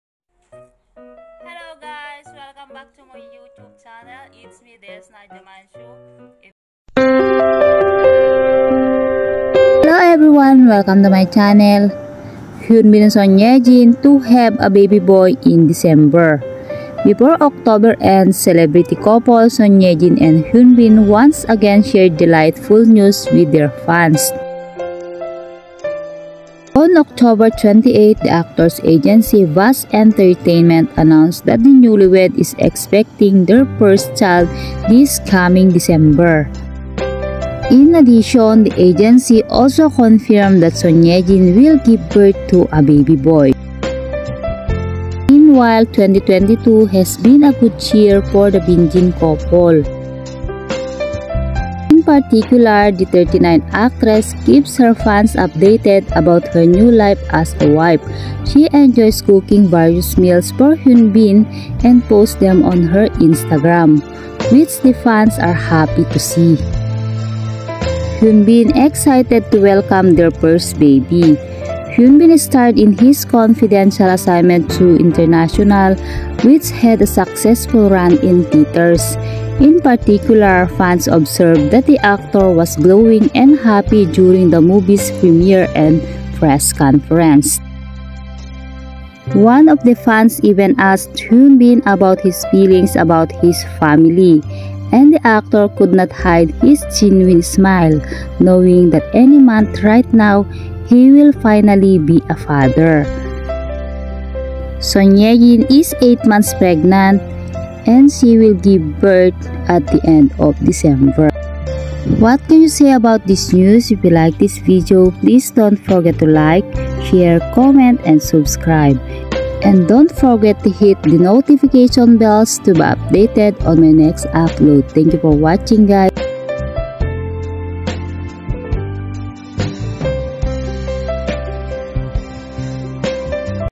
SON YE JIN AND HYUN BIN ARE VERY EXCITED FOR THEIR FIRST CHILD! (BINJIN SPOTTED)
the photos, videoclips, and music  are not mine, I edit this video and used my own voice to convey some exciting news to Hyun Bin and Son YeJin. The owner of the photo in my video is acknowledged and appreciated!  [CREDIT AND BIG THANKS]